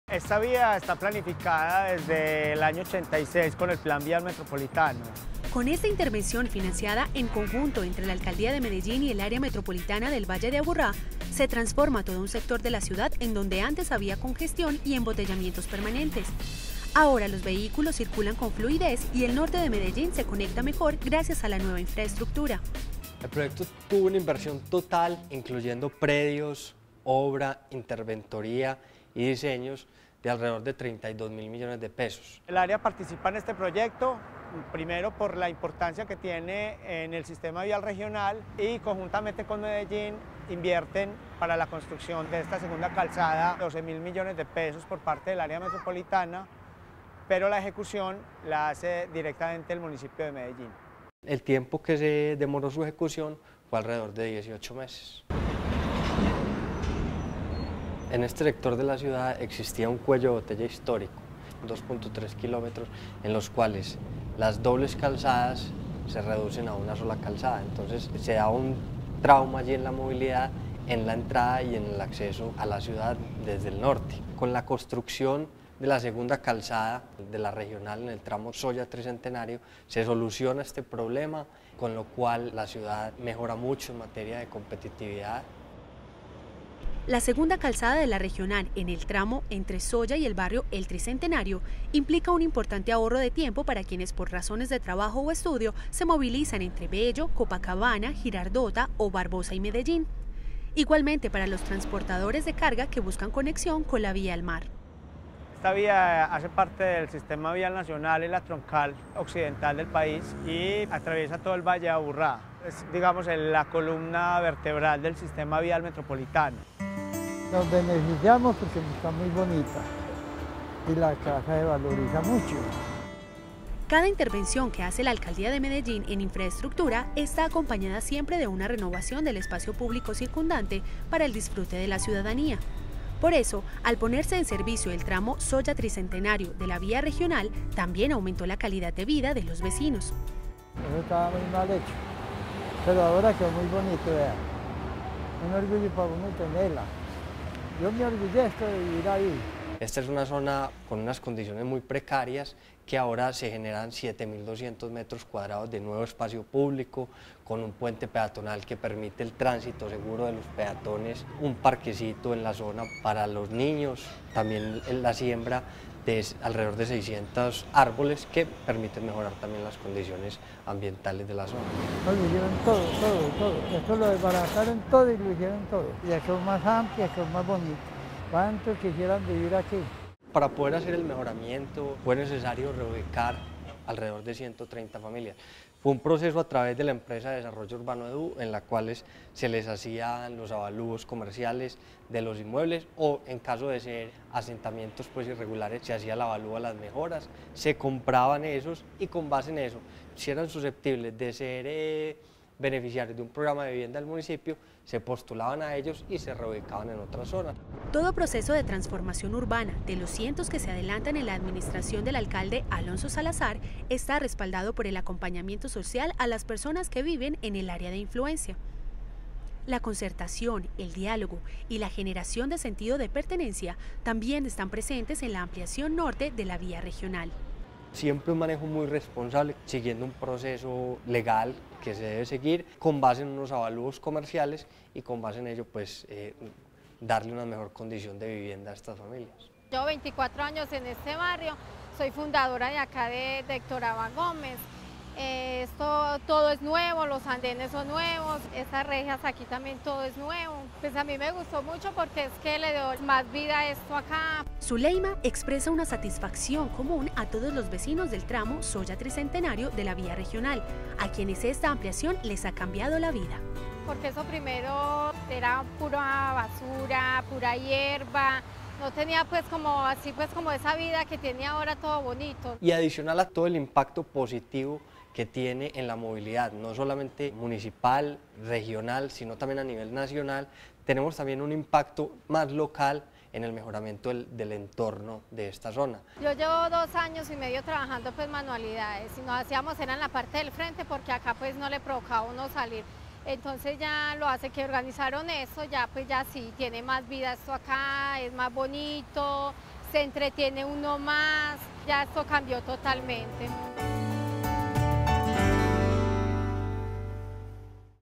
Proyecto vial Solla Tricentenario
Con una inversión de 32 mil millones de pesos, la Alcaldía de Medellín y el Área Metropolitana del Valle de Aburrá ejecutaron este proyecto en tiempo de 18 meses. Un proceso de transformación urbana que beneficia a los habitantes aledaños a la vía  con obras de espacio público.